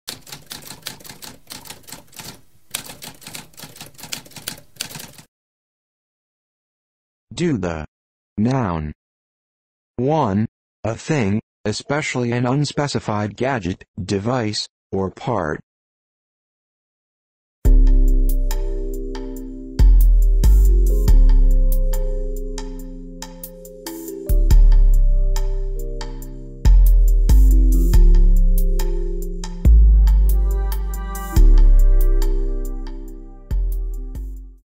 What does doodah mean?
A spoken definition of doodah.

Intro Sound:
Typewriter - Tamskp